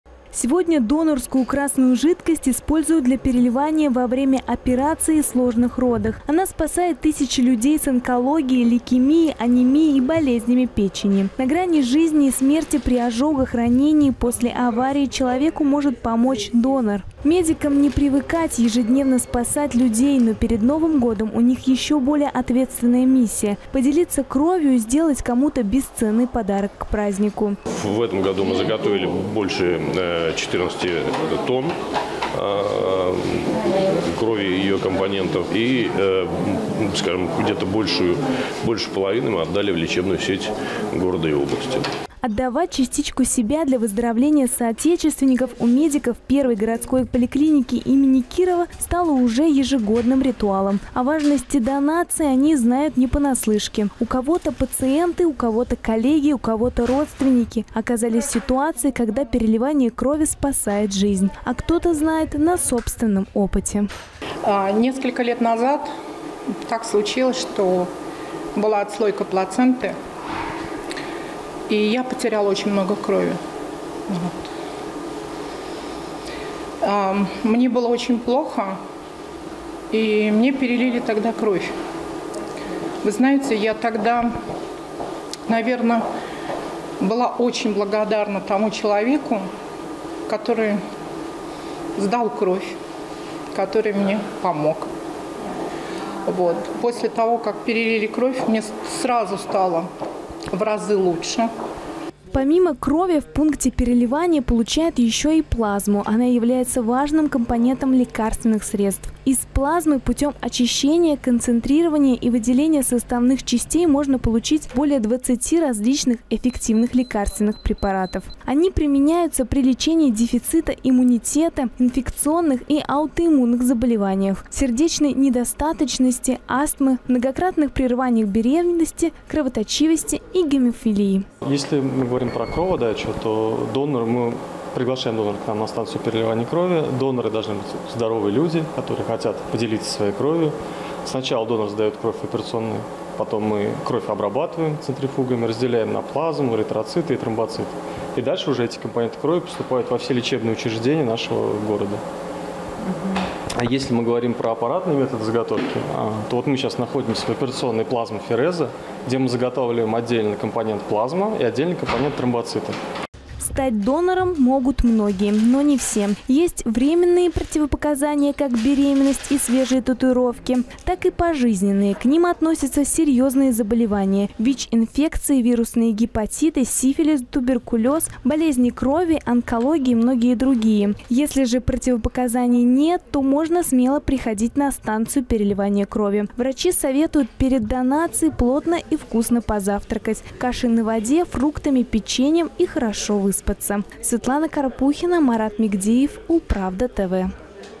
Медики из первой городской поликлиники стали донорами
Донорская кровь ежедневно спасает не один десяток жизней. Ульяновские медработники спасают пациентов ежедневно, в том числе и в качестве доноров.